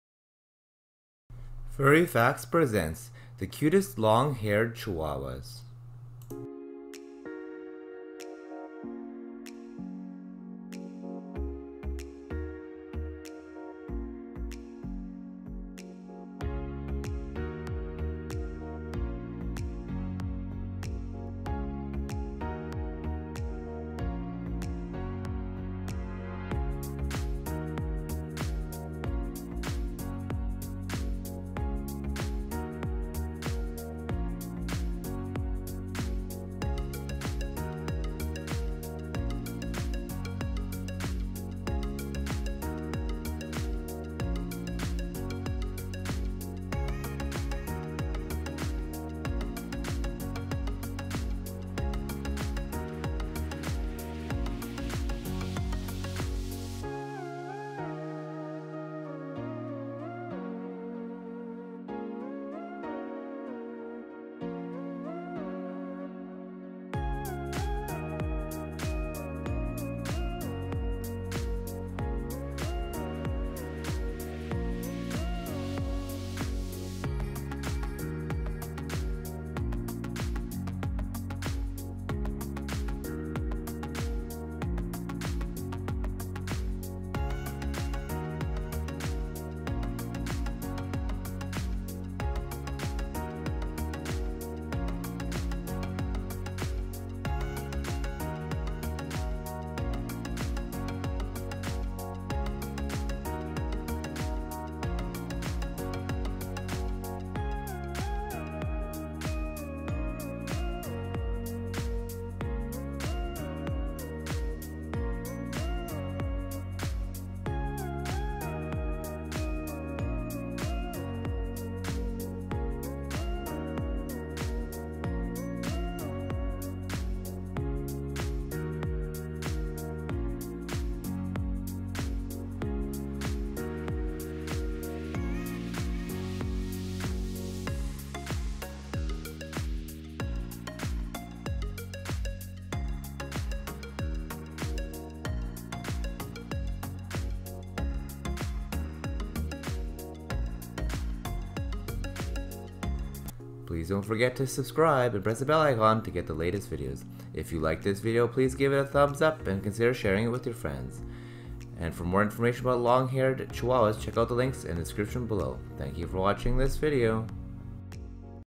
Cutest Long-Haired Chihuahua | Compilation Video 2020
Best Dog Food

If you love long haired chihuahuas then you'll love this super cute compilation video.

- Top Interactive Dog Toys in 2020
- Top 10 Best Dog Puzzles
- Top 10 Best Pet Cameras (2020)